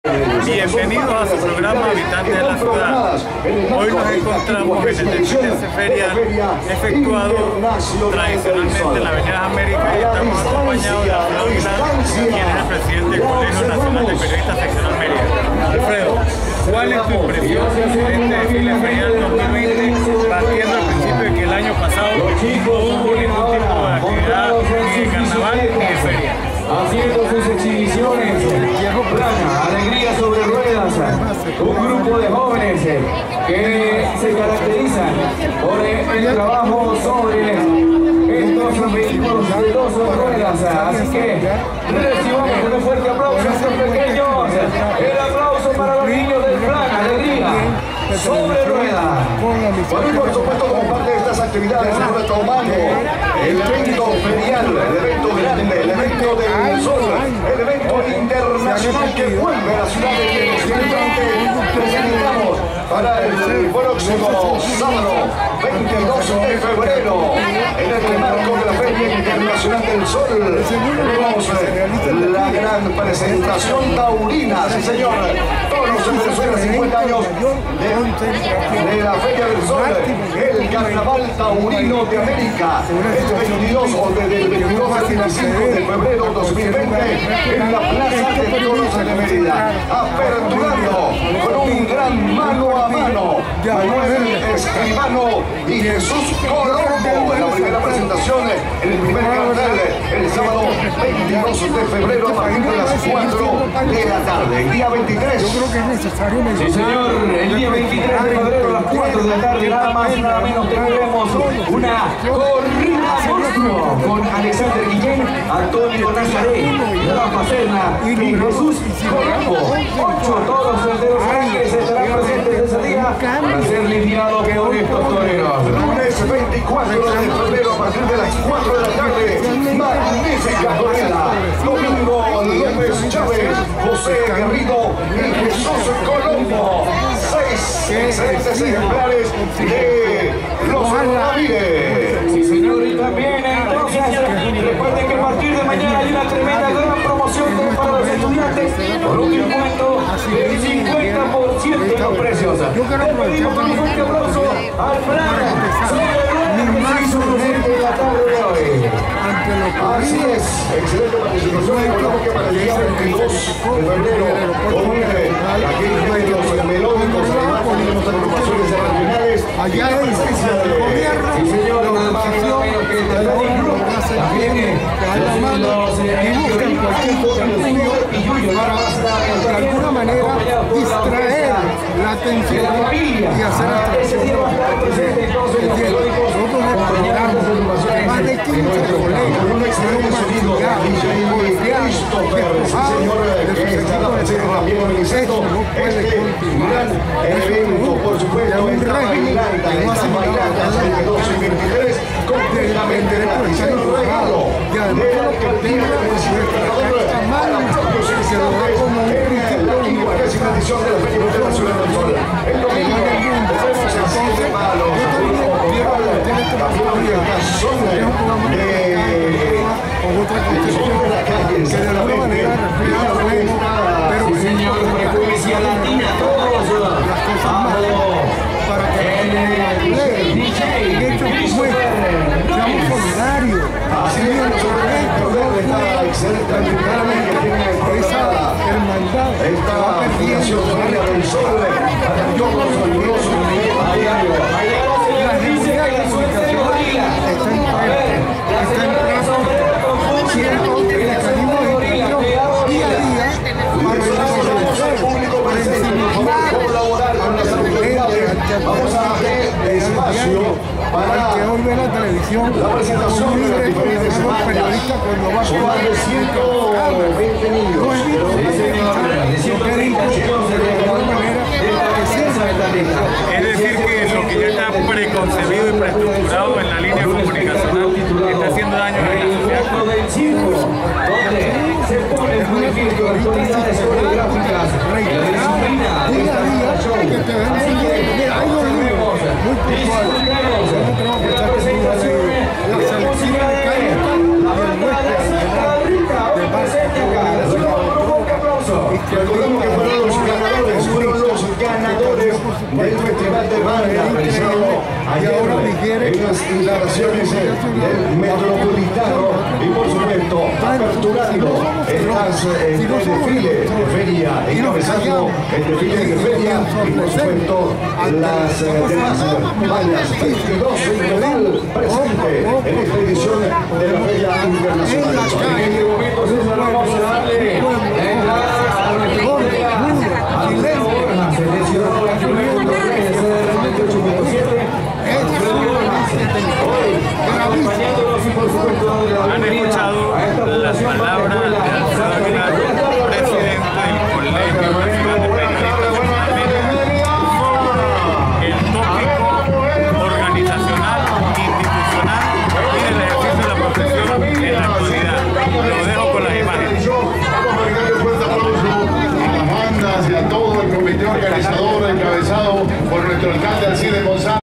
Habitantes de la ciudad: Con Alfredo Aguilar
Bienvenidos al espacio “Habitantes de la ciudad”, busca realzar a esos hombres y mujeres que contribuyen al bienestar nacional. El 16 de febrero de 2020, Jim Morantes, conversó con el Alfredo Aguilar, presidente del Colegio Nacional de Periodistas sobre las impresiones del desfile ferial, efectuado en la Av. Las Américas de la ciudad de Mérida.